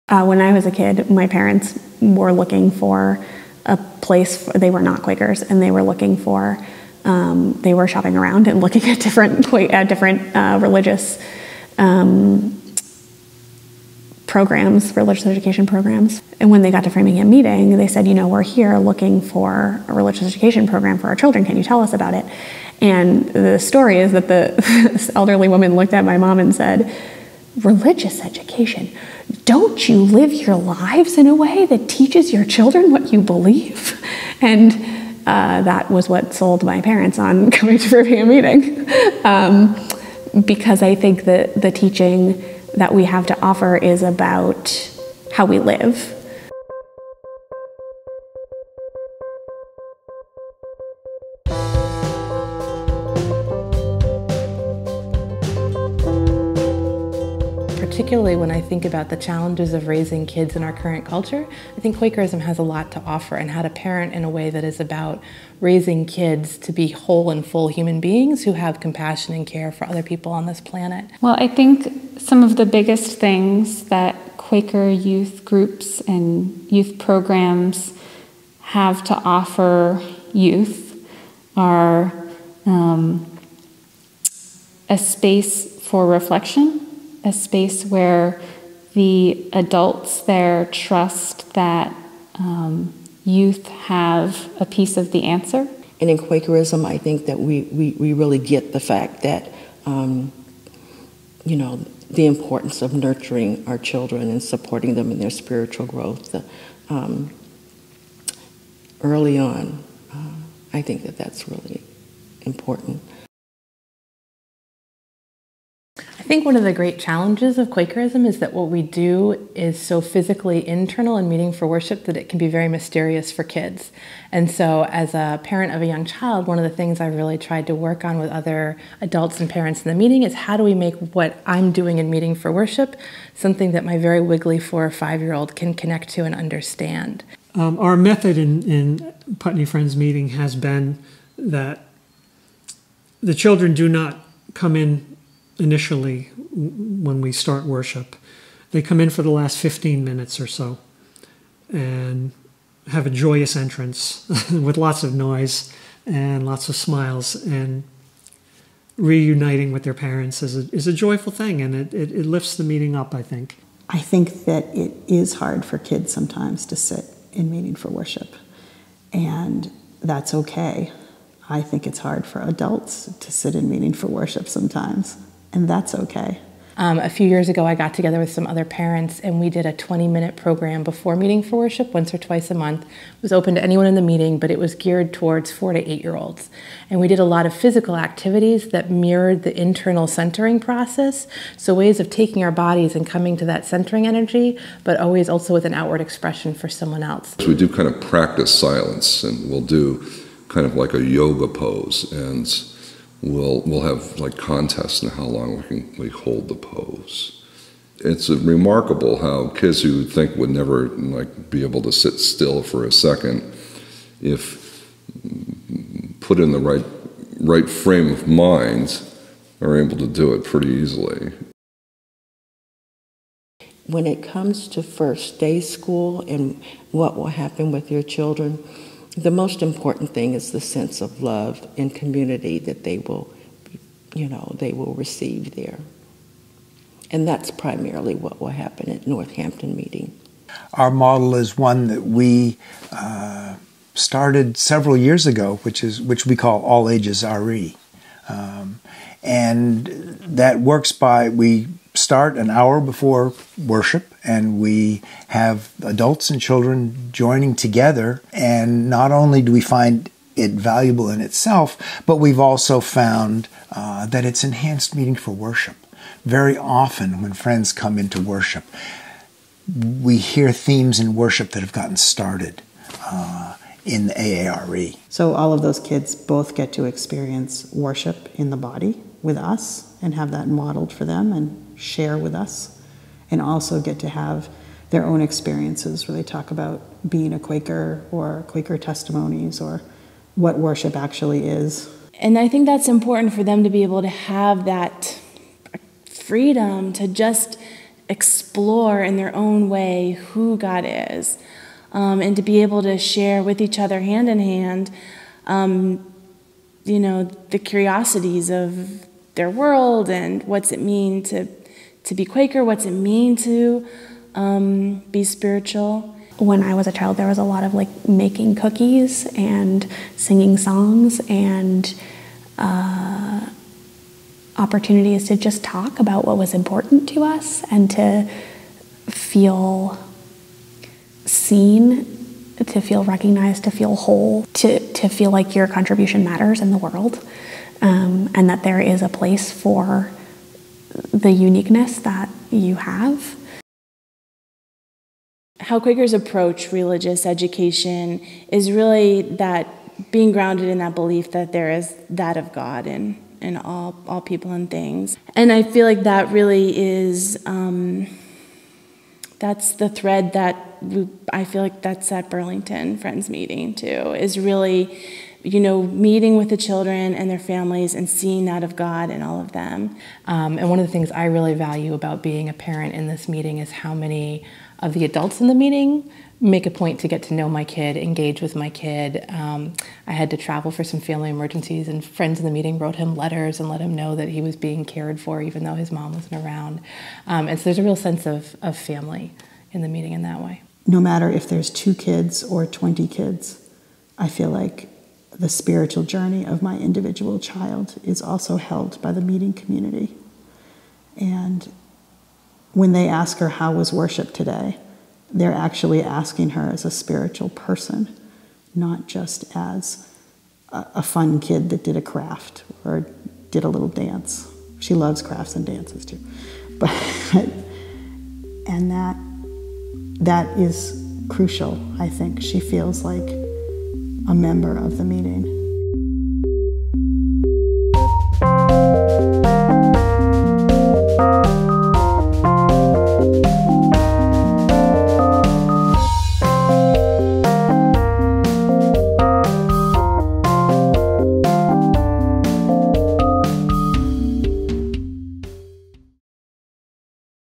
How Do Quakers Support Children's Spirituality?
If you bring your children to a Quaker meeting, what will their experience be?

⇒⇒Transcript:

Honor Woodrow: When I was a kid, my parents were looking for a place—they were not Quakers—and they were shopping around and looking at different religious education programs.  And when they got to Framingham Meeting, they said, “We’re here looking for a religious education program for our children.  Can you tell us about it?”

And the story is that this elderly woman looked at my Mom and said, “Religious education? Don’t you live your lives in a way that teaches your children what you believe?”  And that was what sold my parents on coming to Framingham Meeting, because I think the teaching that we have to offer is about how we live.

How Do Quaker Meetings Welcome Children and Families?

Lisa Graustein: Particularly when I think about the challenges of raising kids in our current culture, Quakerism has a lot to offer on how to parent in a way that is about raising kids to be whole and full human beings who have compassion and care for other people on this planet.

Nia Thomas: Well, I think some of the biggest things that Quaker youth groups and youth programs have to offer youth are a space for reflection, a space where the adults there trust that youth have a piece of the answer.

Angela Hopkins: And in Quakerism, I think that we really get the fact that… the importance of nurturing our children and supporting them in their spiritual growth.  Early on, I think that that’s really important.

Introducing Children to Silent Worship

Lisa Graustein: I think one of the great challenges of Quakerism is what we do is so physically internal in meeting for worship that it can be very mysterious for kids. And so as a parent of a young child, one of the things I really try to work on with other parents and adults in this meeting is: How do we make what I’m doing in meeting for worship something that my very wiggly four- or five-year-old can connect to and understand?

Roger Vincent Jasaitis: Our method in Putney Friends Meeting has been that the children do not come in initially when we start worship.  They come in for the last 15 minutes or so, and have a joyous entrance with lots of noise and lots of smiles.  Reuniting with their parents is a joyful thing, and it lifts the meeting up I think.

Kathleen Wooten: I think that it is hard for kids sometimes to sit in meeting for worship, and that’s okay.  I think it’s hard for adults to sit in meeting for worship sometimes, and that’s okay.

Lisa Graustein: A few years ago I got together with some other parents and we did a twenty-minute program before meeting for worship once or twice a month.  It was open to anyone in the meeting, but it was geared toward 4 to 8 year olds, and we did a lot of physical activities that mirrored the internal centering process, so ways of taking our bodies and coming to that centering energy, but always with an outward expression for someone else.

Doug Lippincott: We do kind of practice silence, and we’ll do kind of like a yoga pose and we’ll have contests on how long we hold the pose.  It’s remarkable how kids you would think would never be able to sit still for a second, if put in the right frame of mind, are able to do it pretty easily.

The views expressed in this video are of the speakers and do not necessarily reflect the views of Friends Journal or its collaborators.